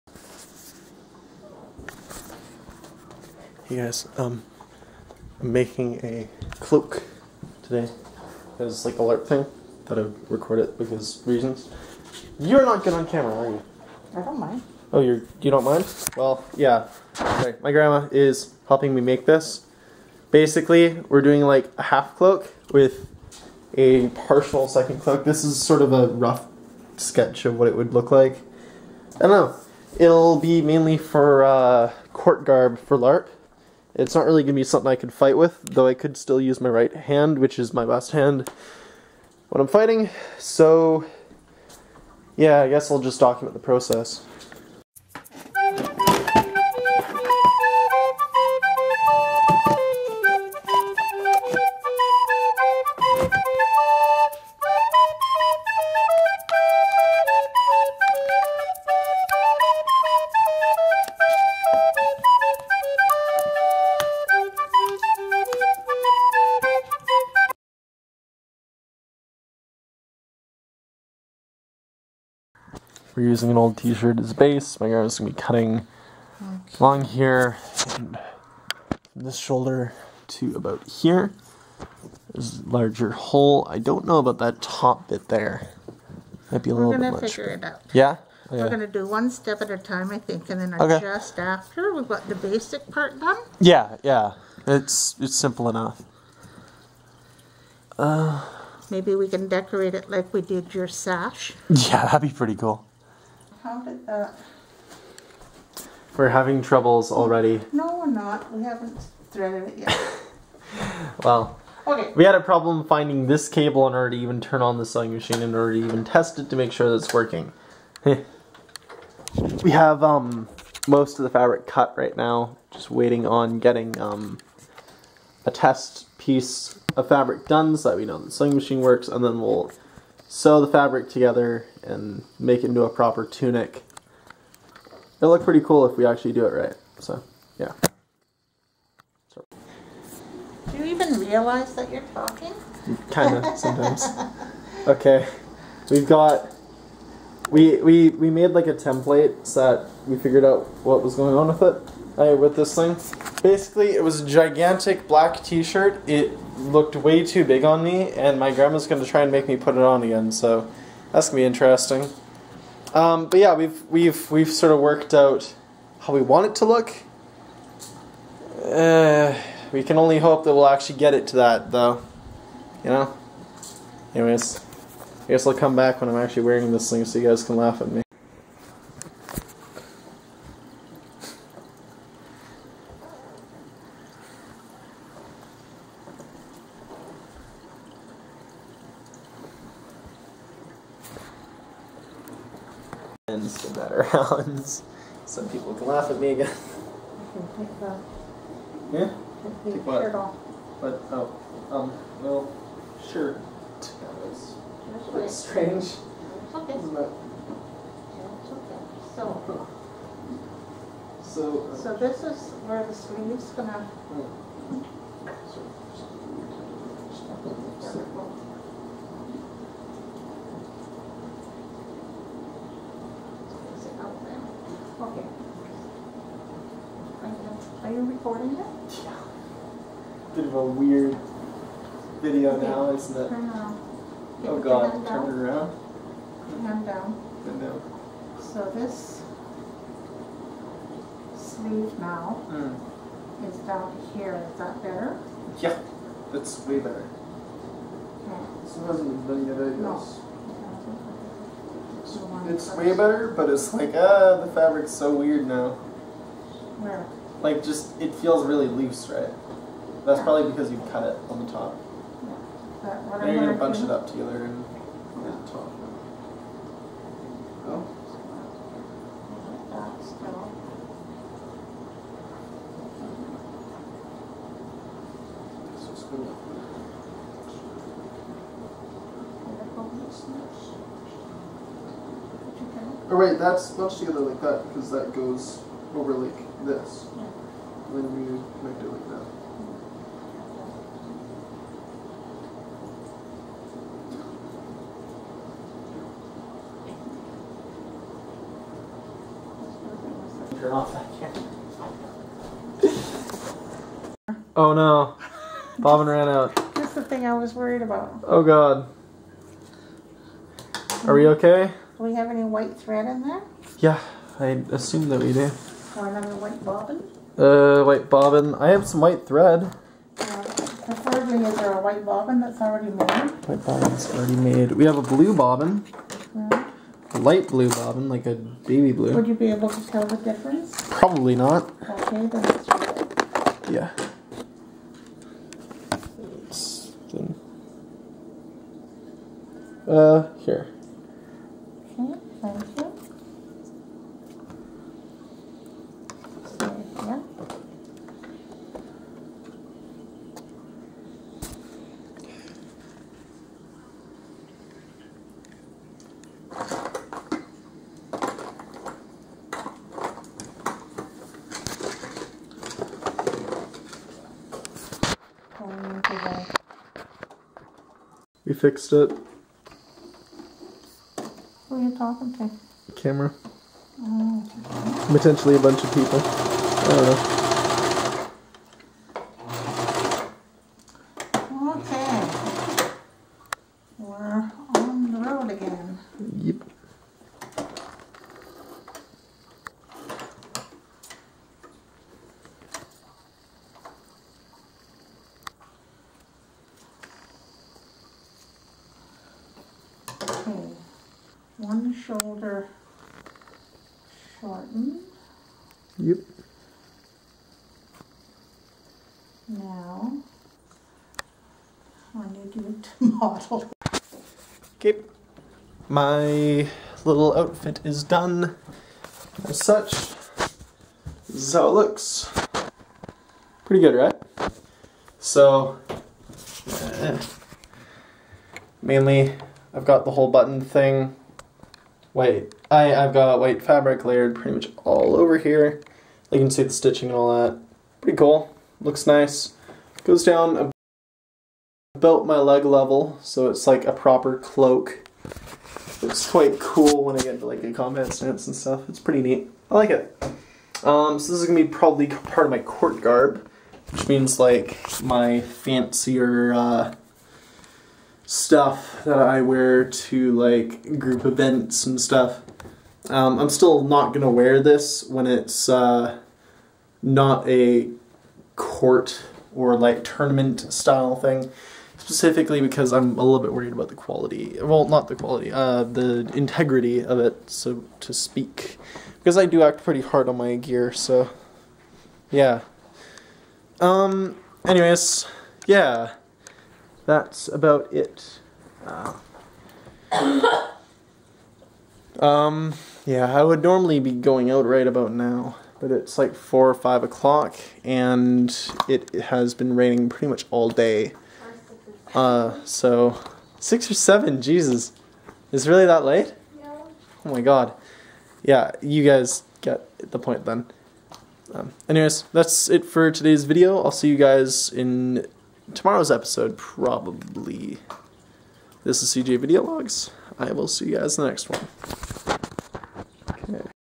vlog 27: i make a jacket for larp with my grandma and somehow dont mess up!
this was a ton of editing in the video editor that youtube has, and what made it horrid is i was getting barely 3-5 fps the entire time. ahhhhhh.